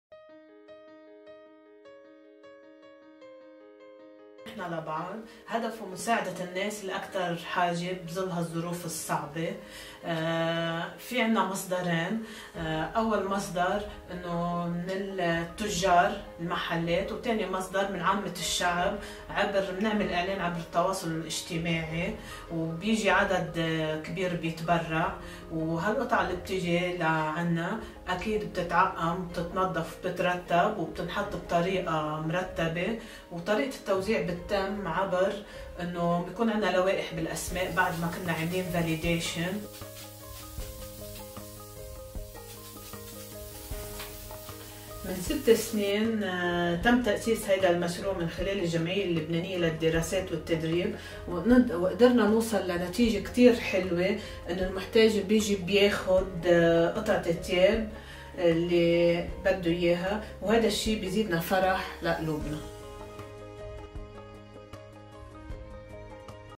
Nehna la baad ramona yaghy
Ramona yaghi, supported by LOST, started a clothes donation center for the poor and the needy.